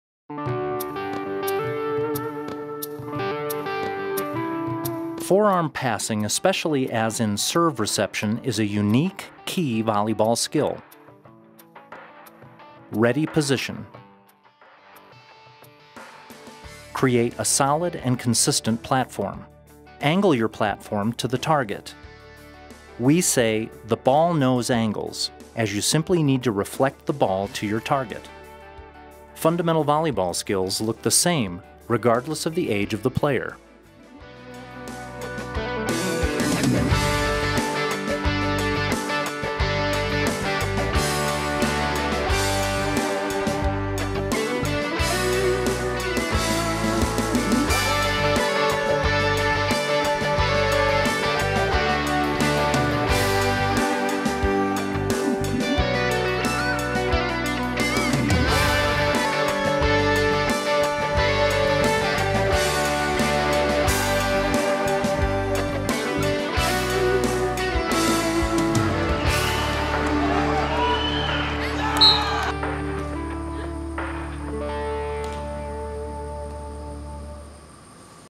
USAV Skill Video Forearm Passing
U.S. National Team members Kawika Shoji and Caitlyn Donahue provide forearm passing tips designed for all ages.